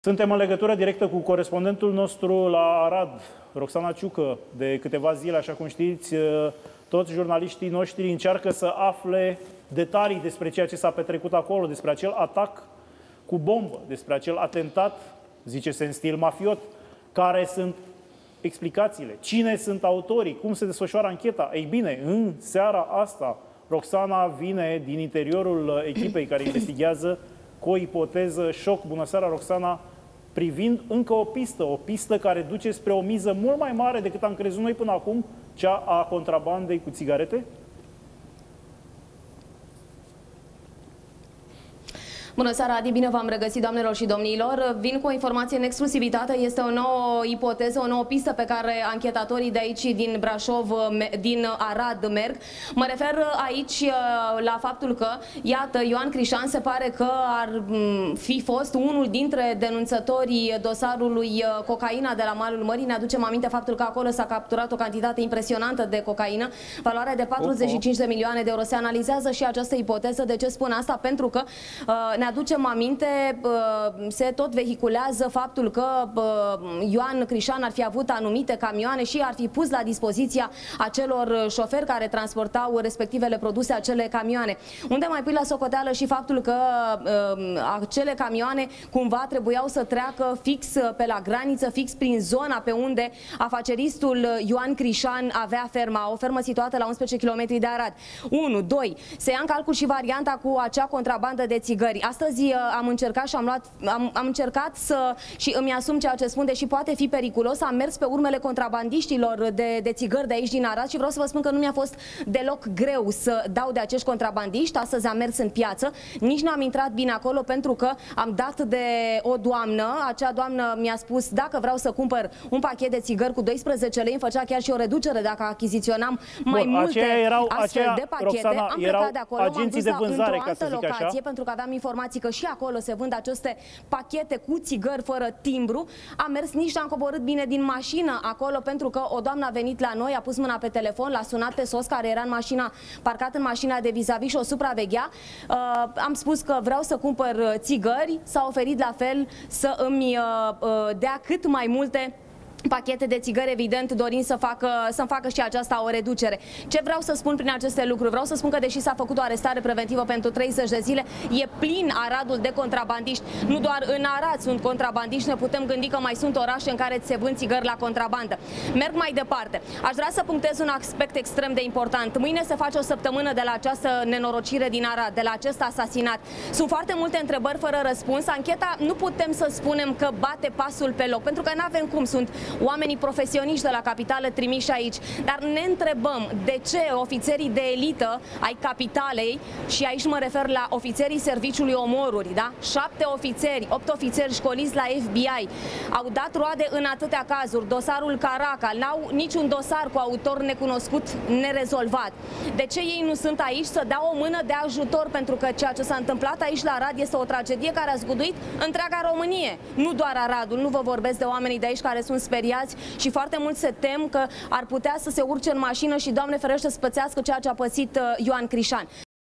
De ce a fost ţintă Ioan Crişan? O nouă pistă
Roxana Ciucă a prezentat în exclusivitate la Antena 3 o nouă pistă pe care merg anchetatorii în cazul asasinatului de la Arad în care omul de afaceri Ioan Crişan şi-a pierdut viaţa în cel mai îngrozitor mod cu putinţă.